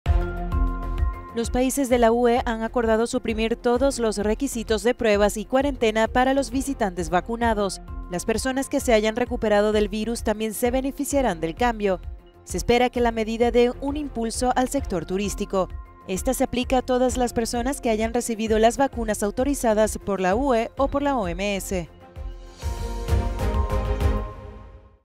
Los países de la UE eliminan la prueba COVID y la cuarentena como requisitos para los viajeros
Las personas que se hayan recuperado del virus también se beneficiarán de la medida.